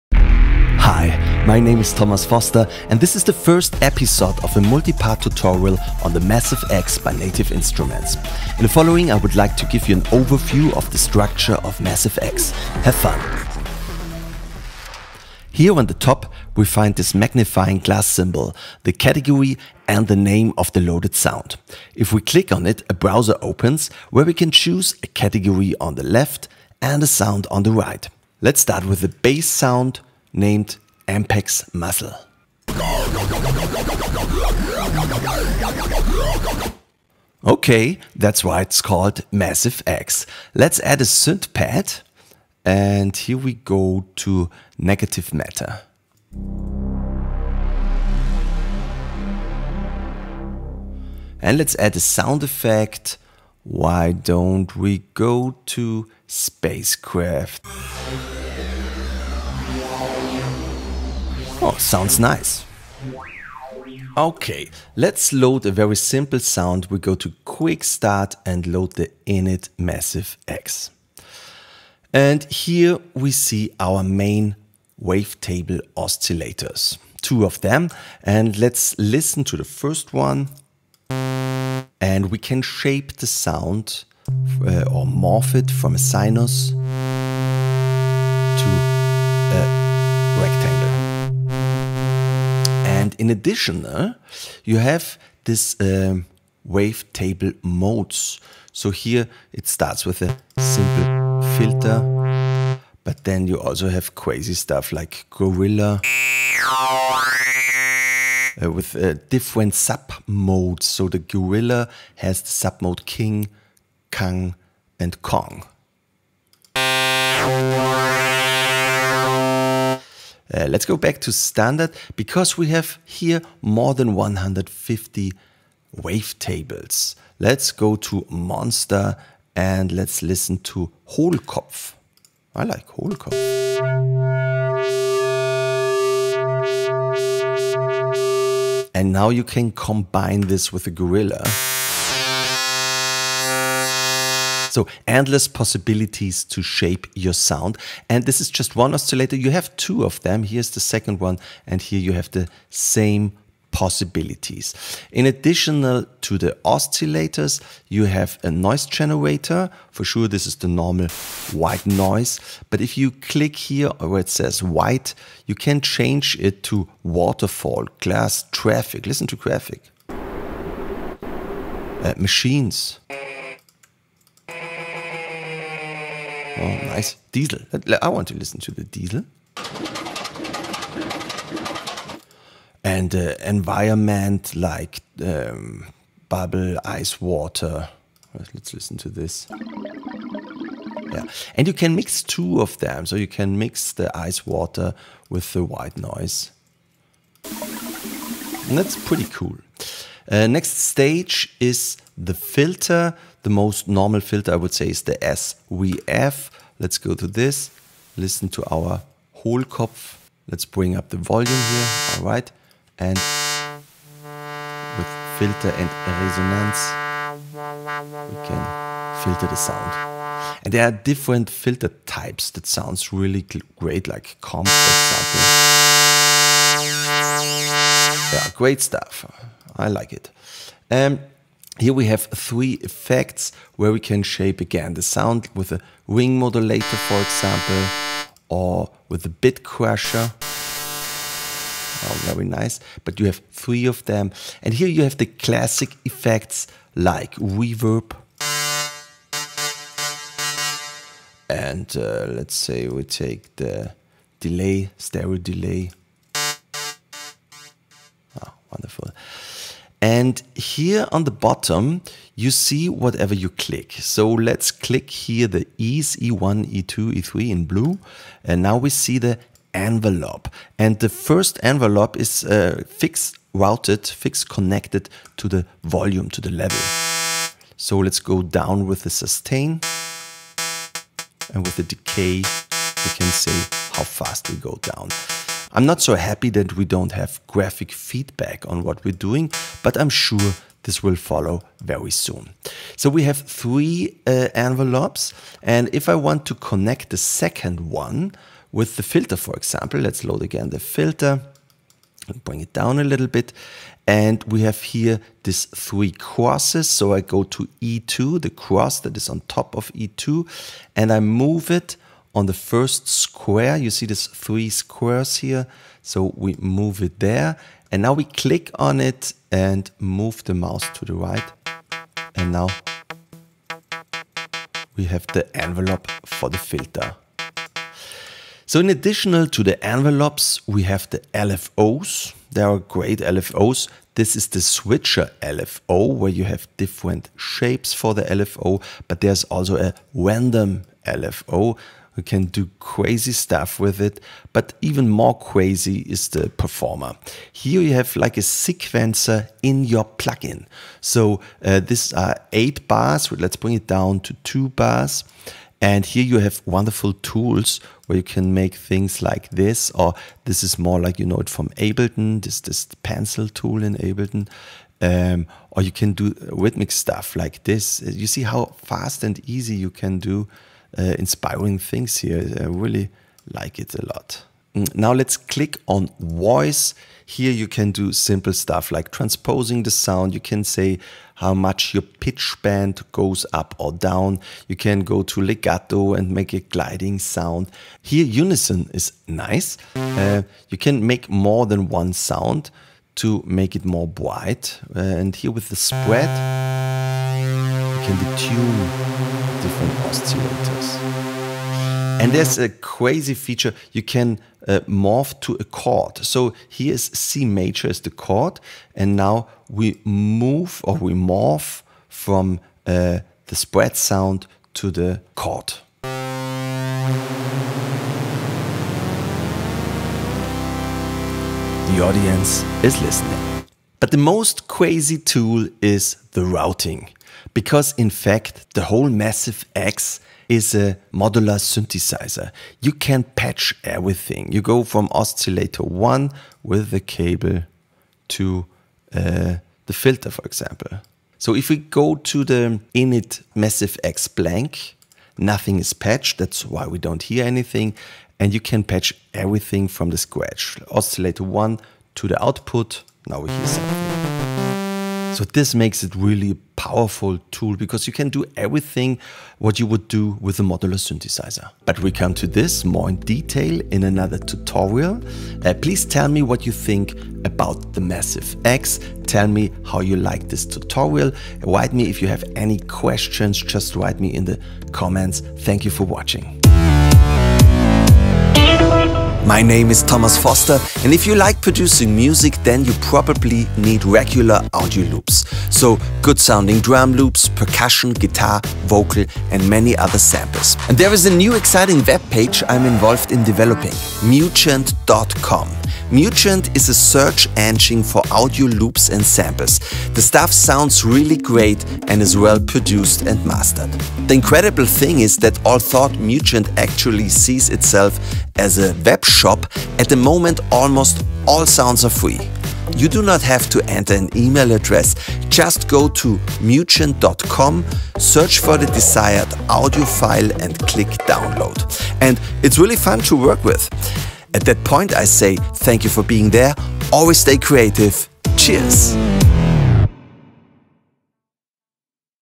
Massive X #1 Tutorial - Native Instruments
Massive X # 1 Tutorial German - Native Instruments

This is the first part of a multi-part tutorial for Massive X by Native Instruments. In the first episode I'll show you the Dual Wavetable, the Noise Oscillator, the Filter, the Effects, the Switcher LFO, the Random LFO, the Standard Envelope, the Preformance Mode, as well as the routing of the Massive X.
Massive X is a VST plugin from the "Komplete 12" Bundle.